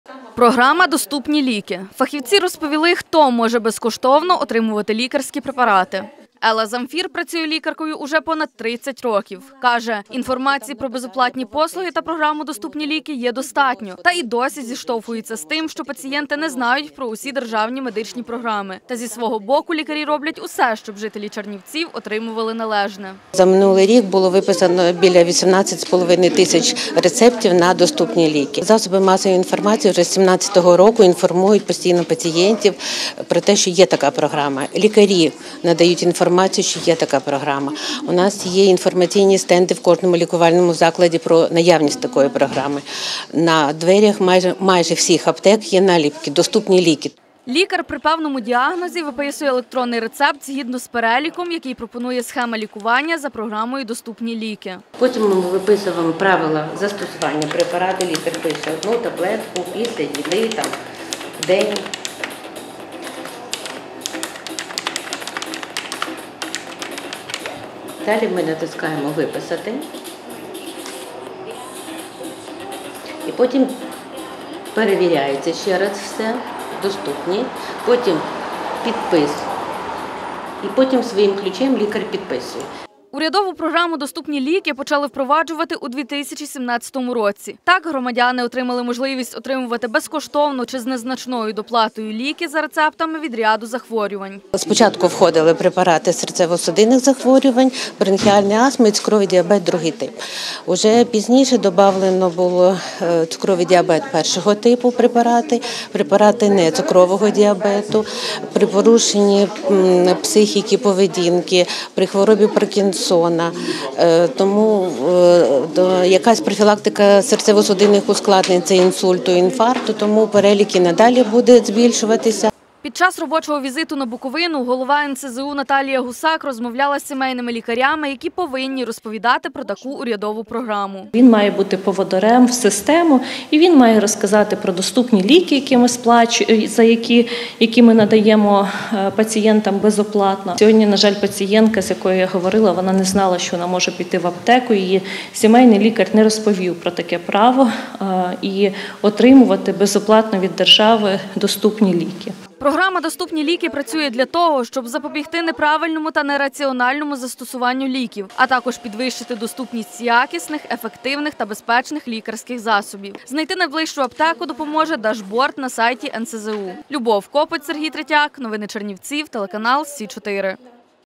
Програма “Доступні ліки”: хто і як може безкоштовно отримувати лікарські препарати | С4
Фахівці розповіли хто може безкоштовно отримувати лікарські препарати за програмою «Доступні ліки».